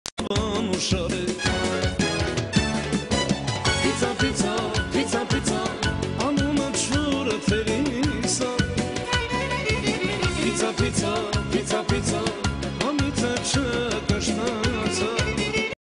How To Get A Pizza for FREE !! TUTORIAL
Stealing a Pizza lika a Boss. Prank Fool Funny